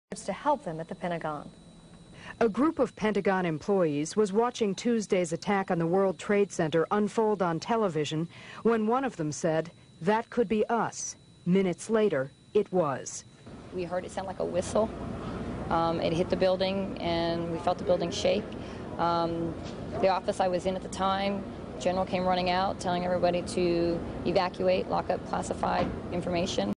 9/11 - Pentagon Employee Eyewitness Heard What Sounded Like A Whistle Before The Explosion
9/11 - Pentagon Employee Eyewitness Heard What Sounded Like A Whistle Before The Explosion